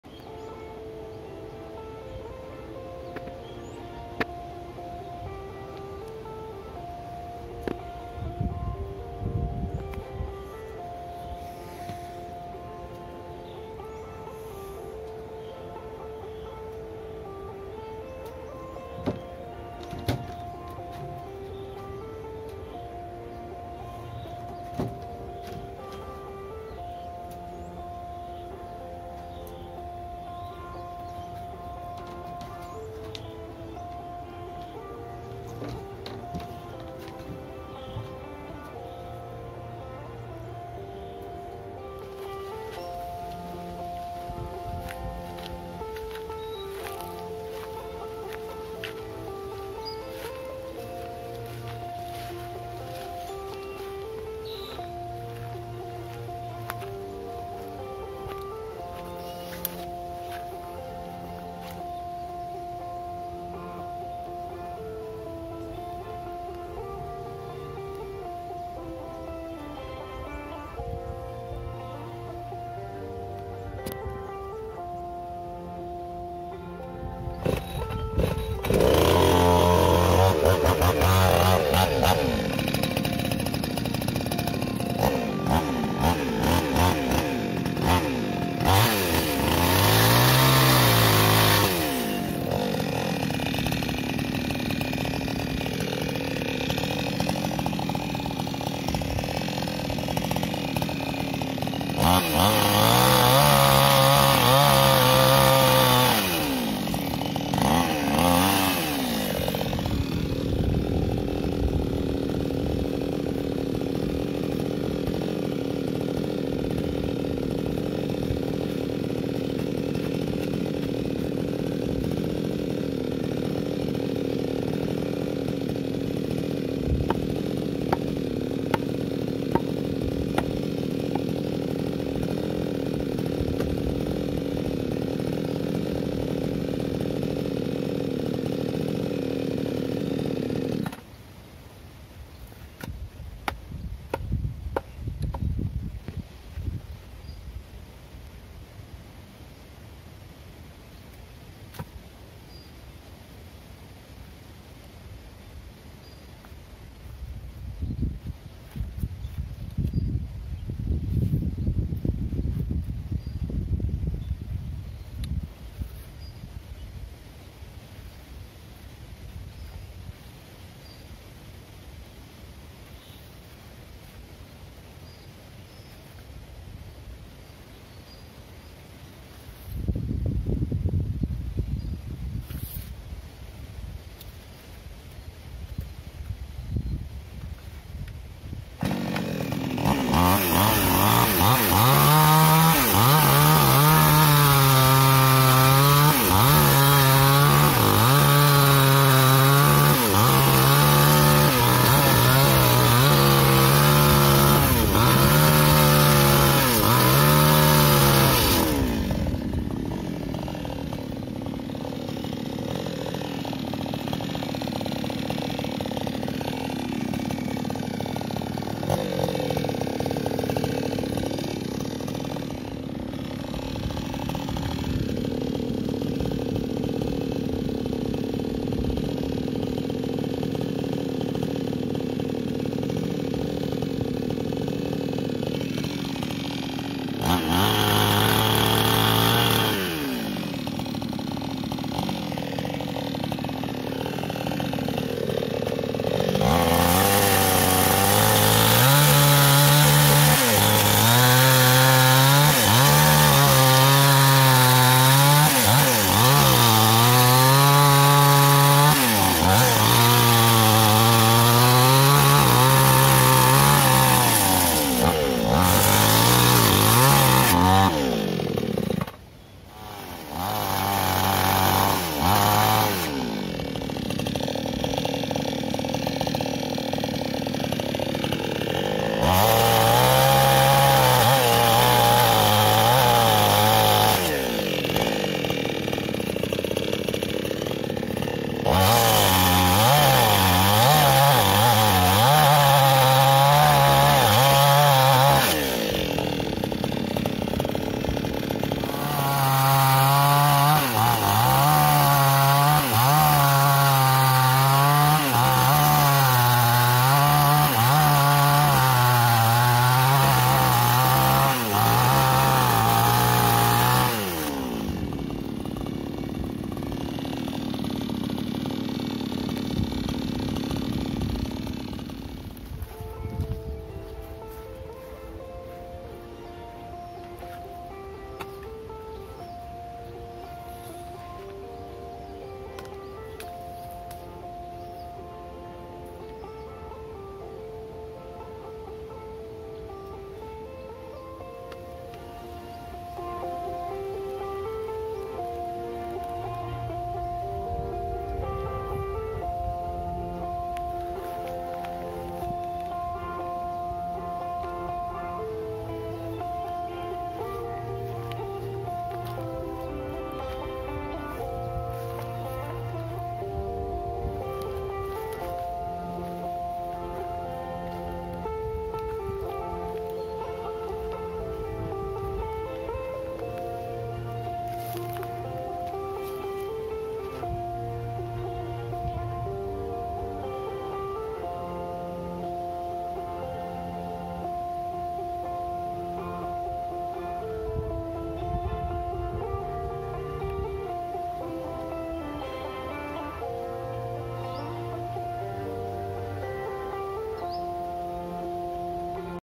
Buat pondok kebun kelapa: cari batang kelapa untuk ganti papan lantai sementara beranda yang reput
Assalamualaikum dan salam sejahtera semua sahabat terbaik ku di channel ILHAM STATION, hari ini cari batang kelapa untuk tukar papan lantai sementara beranda hadapan pondok. apabila batang kelapa sudah ada, saya potong kepada 6 bahagian dengan 2 size berbeza , kerja potong papan kena di tangguhkan atas sebab kecuaian semasa mengangkat barang kelapa ke atas galang[alas]. slip disc berlaku lagi di bahagian yang sama. tambah dengan ubat yang tidak ada. saksikan video ini sehingga selesai agar tidak berlaku salah faham. terima kasih atas kehadiran sahabat terbaik ku di sini.